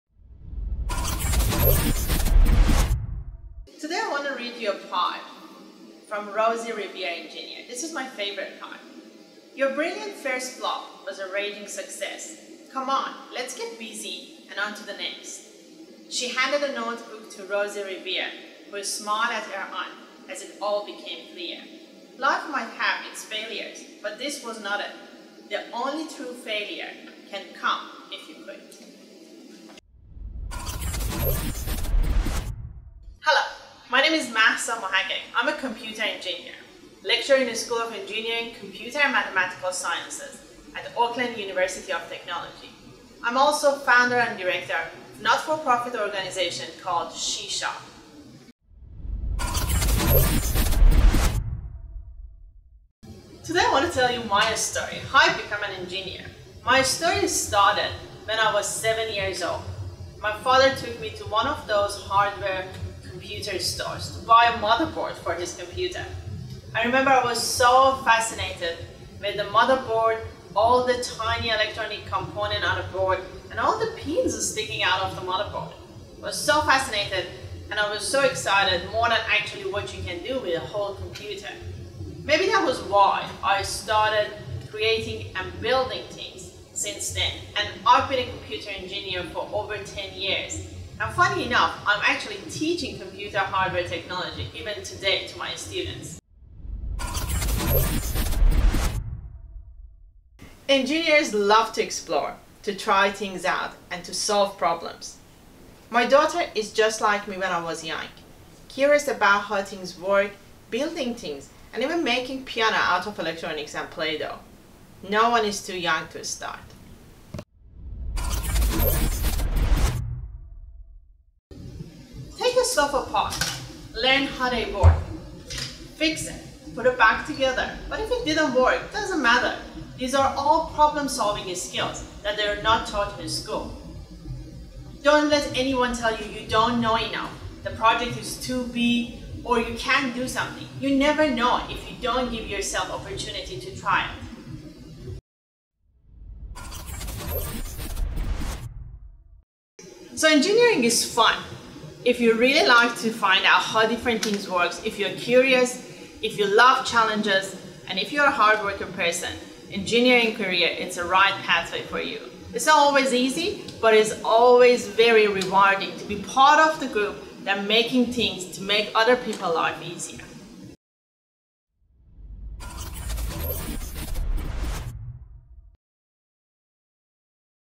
FindingRosieNZ :Dr Mahsa Mohaghegh
To celebrate International Women in Engineering Day we are telling the stories of the amazing women in engineering in New Zealand with the help of Rosie Revere, Engineer. Dr. Mahsa Mohaghegh is a computer engineer with a background in artificial intelligence and natural language processing. Since 2013, Mahsa has been involved with Google’s Computer Science for High Schools initiative, and has run these workshops in Auckland for the last 4 years. Mahsa is also the founder and director of the women’s technology group She# - shesharp.co.nz, a platform aimed at promoting STEM to the next generation, and creating networking opportunities for tertiary students and industry professionals in the digital sector.
Her passion for promoting careers in technology to young women has been widely recognised, and she has received a number of nominations and awards for her efforts, including the Emerging Leader Award at the NZ Women of Influence Awards, and is also a semi-finalist for New Zealander of the Year 2018.